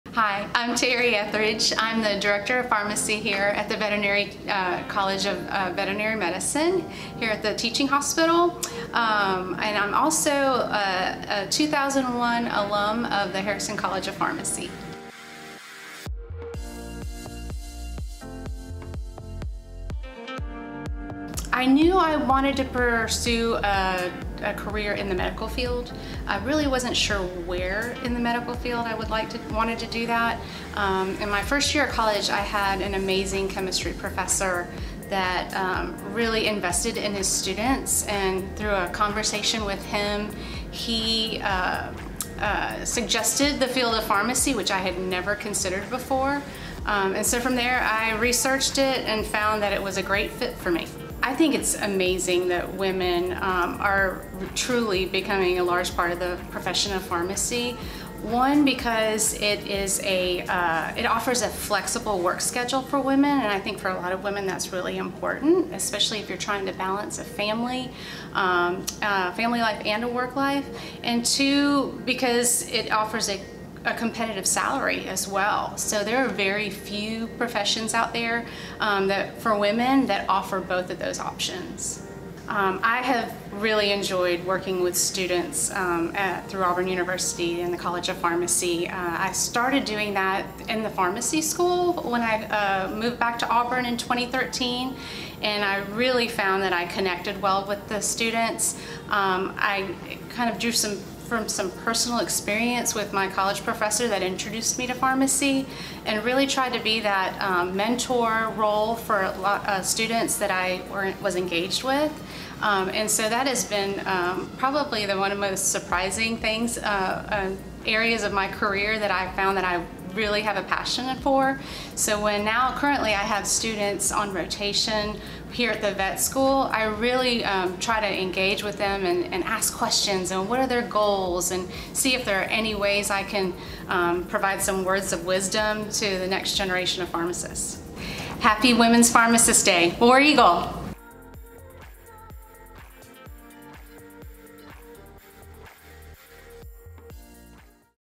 American Pharmacists Month: Teri Etheredge '01
Hear from Harrison College of Pharmacy alum Dr. Teri Etheredge as she talks about what led her to a profession in pharmacy, Women Pharmacists Day, and working with students.